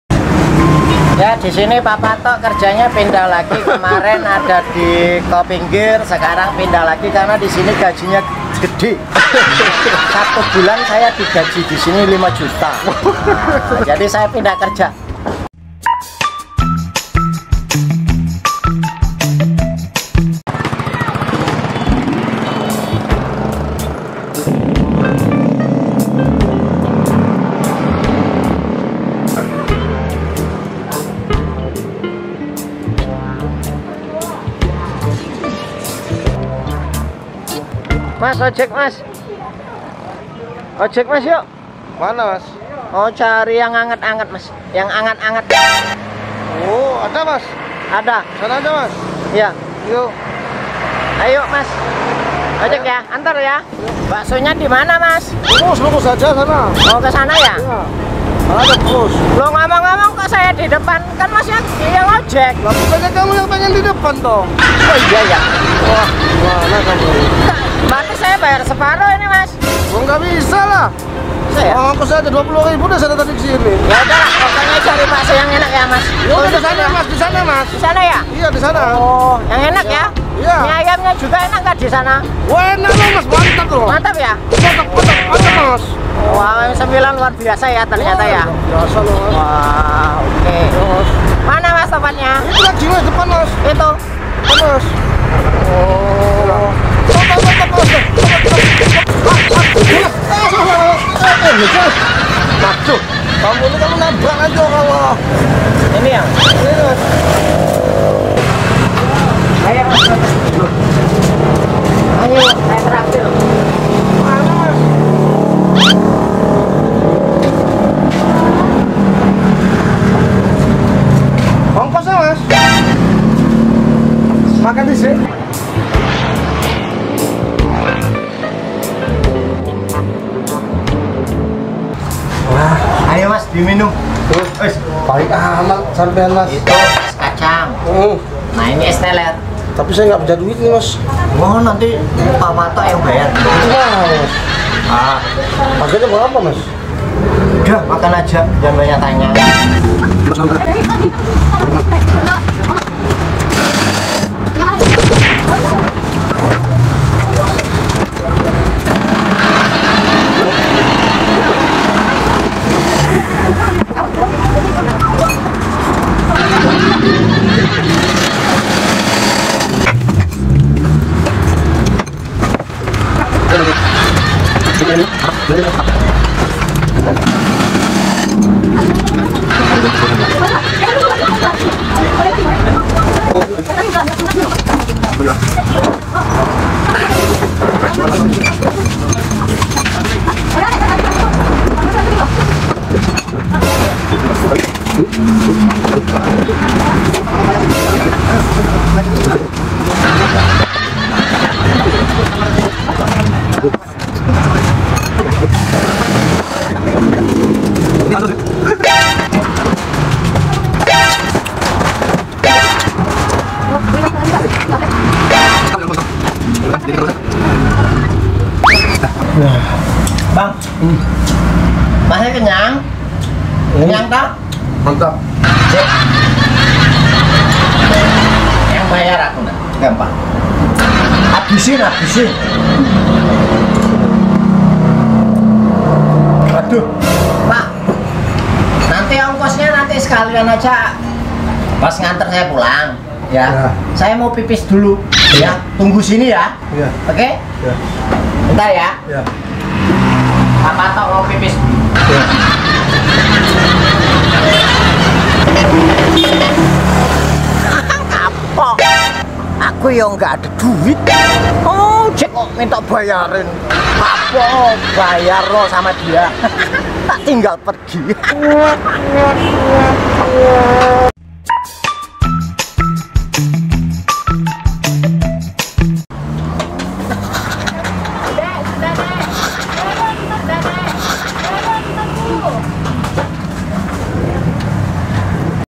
sketsa lucu - Papa Atok buat masalah lagi
papa atok kali ini membuat masalah lagi. kali ini papa atok membuat masalah dengan O.M.9 sehingga masalah ini bakal berbuntut panjang .. ayo kita tonton ya cap sketsany